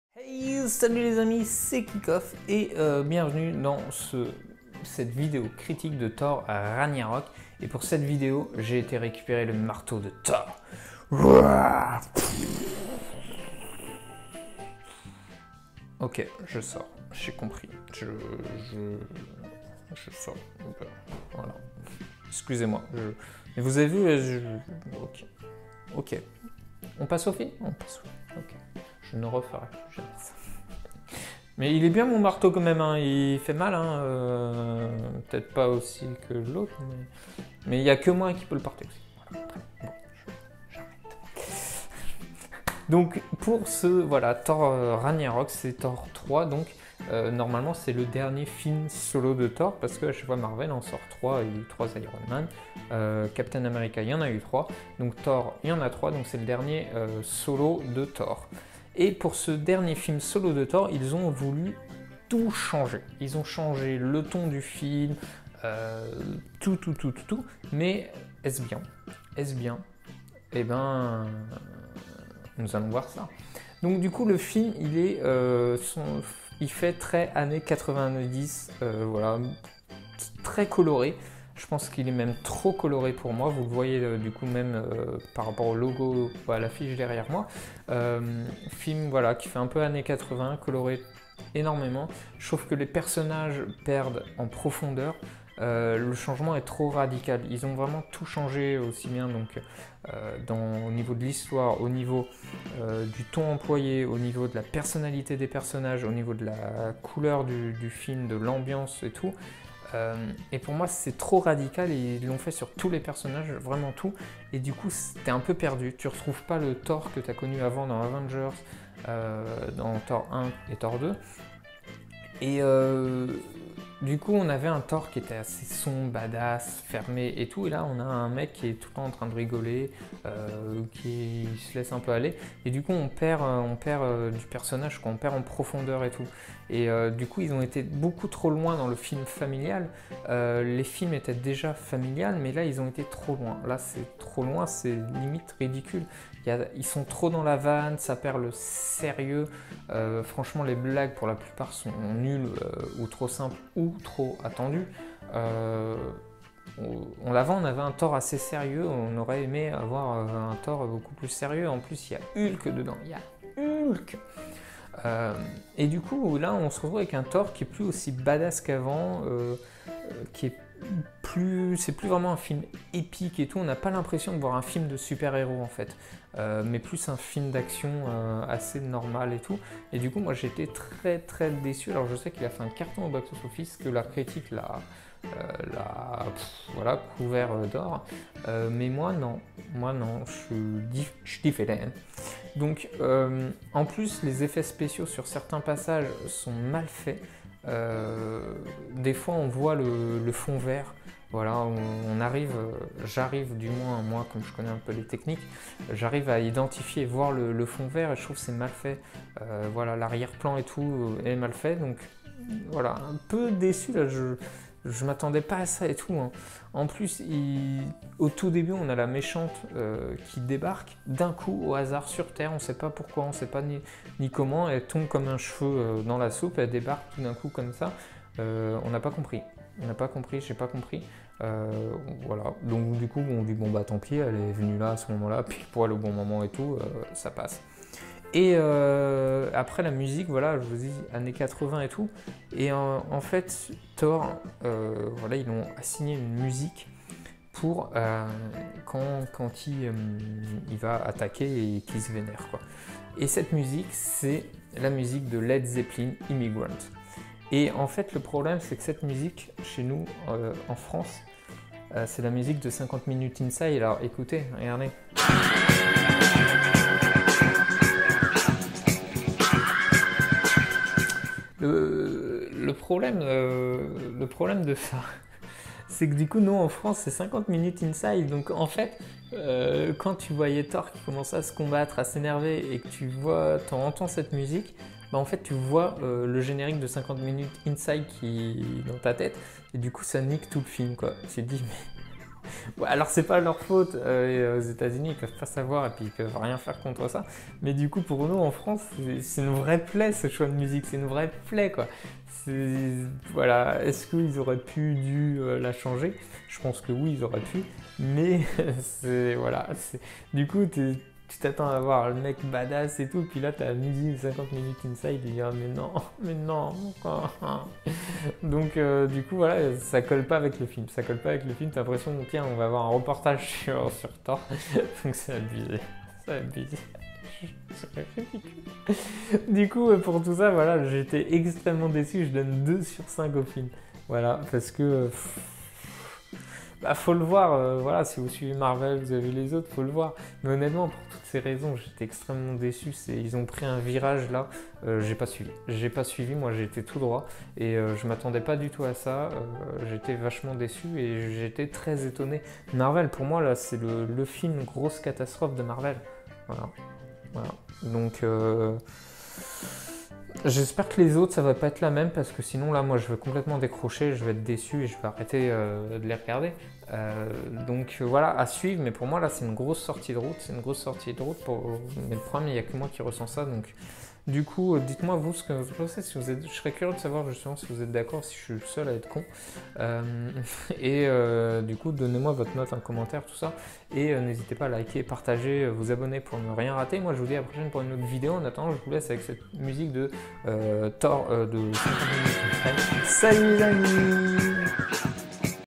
Thor 3 : J'ai osé tenter une caricature de Thor
Il est là ! Ca y est !

Le Dieu du tonnerre is back pour son dernier film solo avec un virage à 380 degrés !

Est-il le dieu du box-office ?

Illustrations :
- Thor Ragnarok / Marvel / Disney